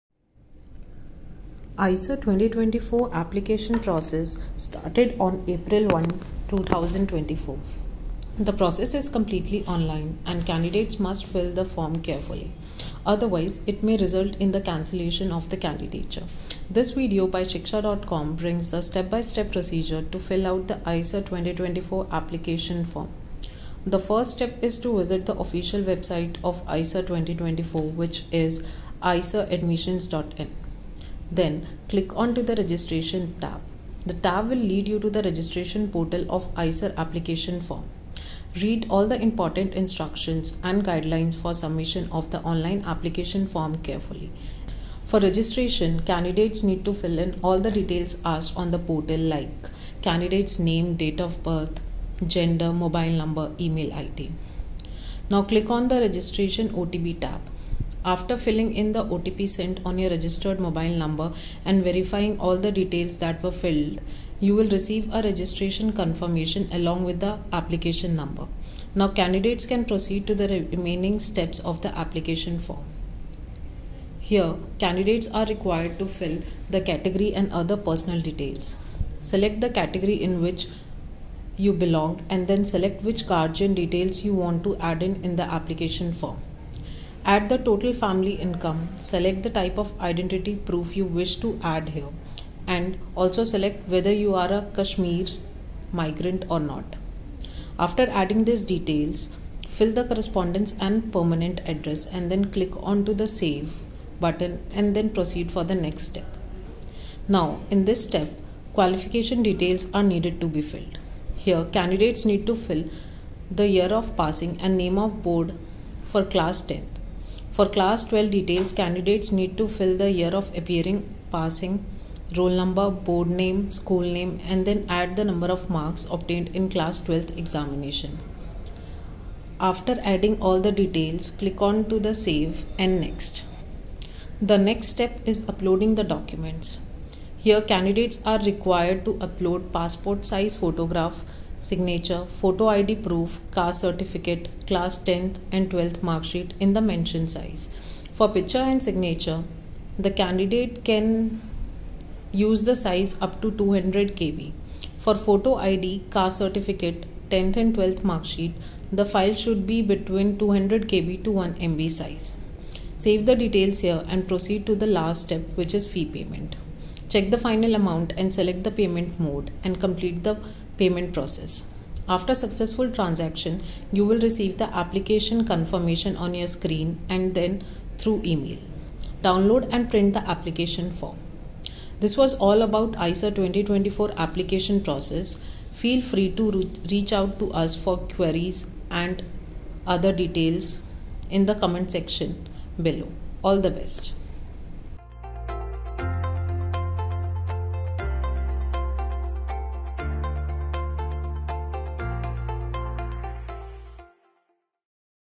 How to Fill IISER Entrance Exam 2024 Application form
IISER Entrance Exam 2024 online registration has been started on April 1, 2024. The deadline to complete the basic registration is May 13, 2024. This video by Shiksha.com guides you step by step on how to complete IISER Entrance Exam 2024 basic registration for July academic session.